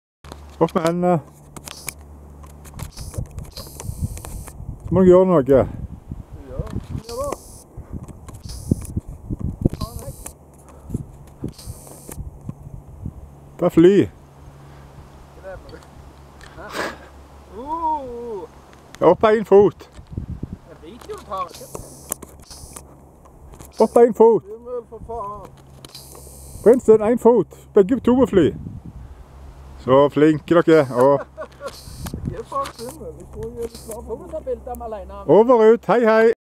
THE PRINS SIGVE ON THE KING HARALD IS LEARNING TO FLAY
Skårasalen er visstnok stedet folk har fått for seg at flyr du ikke herfra så flyr du aldri. Dere kan vel tenke at dette ble tilfellet for disse to kongelige festeløvene her også. Dere har nok med og fote dere, la andre ta seg av dette med å lette fra bakken. PS - SE SÅ LYDIGE DEM ER DA. Gjør alt jeg ber dem om. Snopeguttene